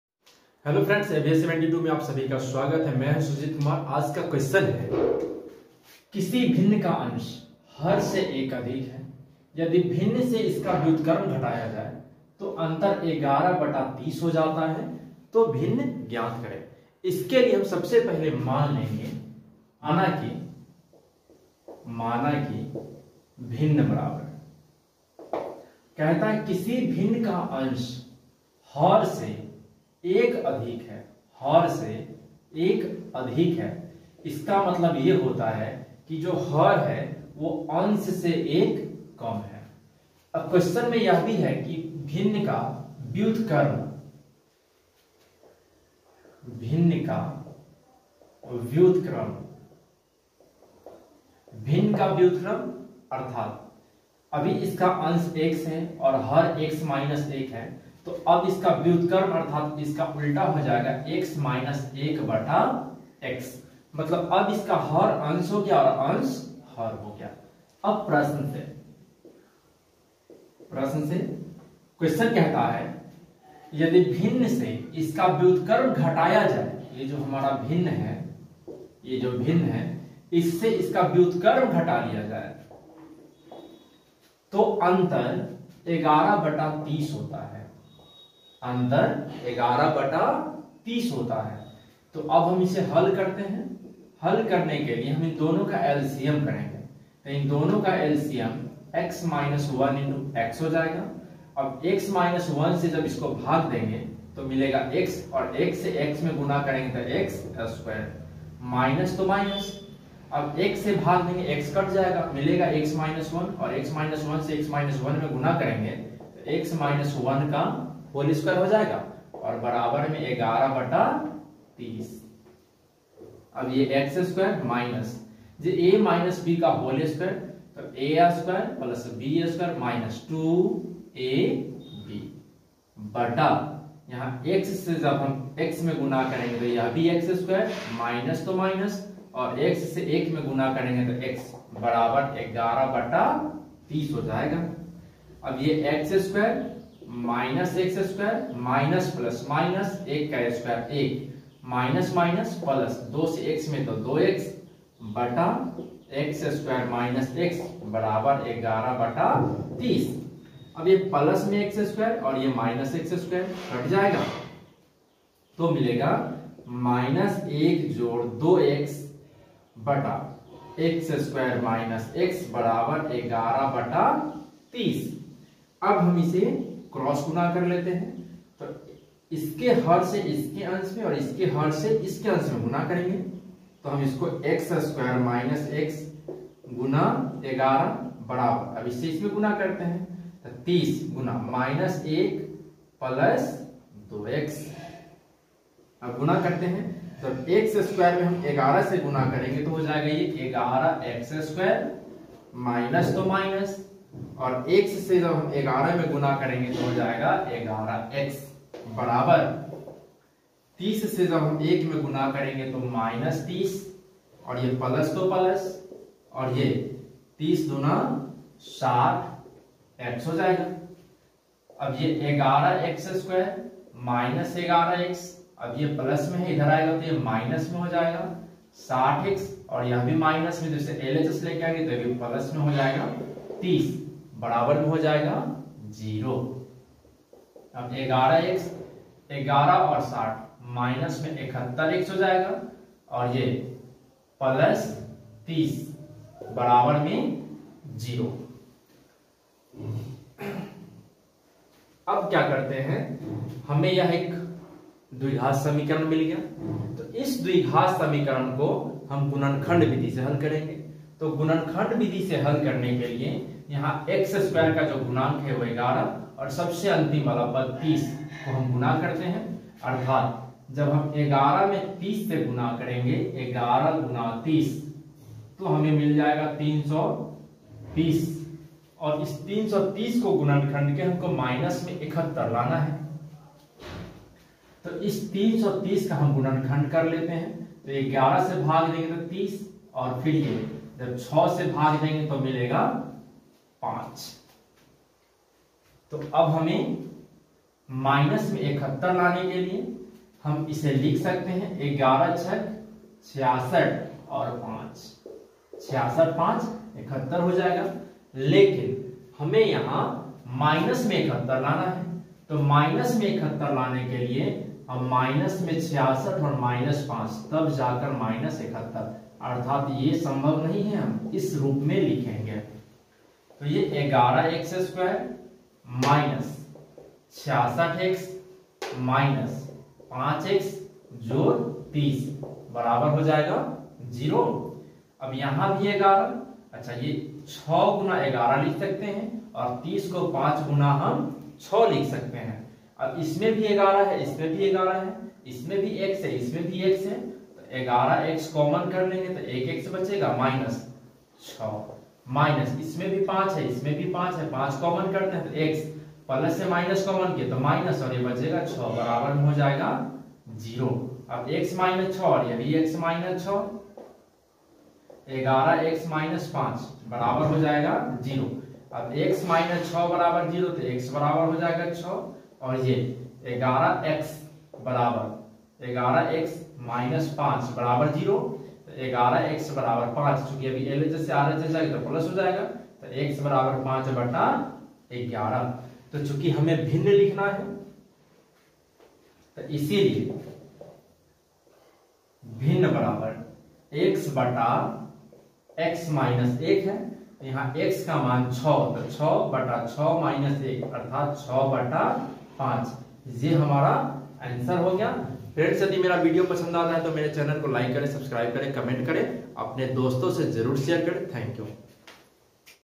किसी भिन्न का अंश,हर से 1 अधिक है यदि भिन्न से इसका व्युत्क्रम घटाया जाए तो अंतर 11 बटा 30 है भिन्न
yadi Kisi bhinn ka Ansh har se Ek Adhik Hai yadi fibhinn es ka vyutkram ghataya Jaaye to Antar 11 Bata 30 ho hata hai bhinn  gyat Karen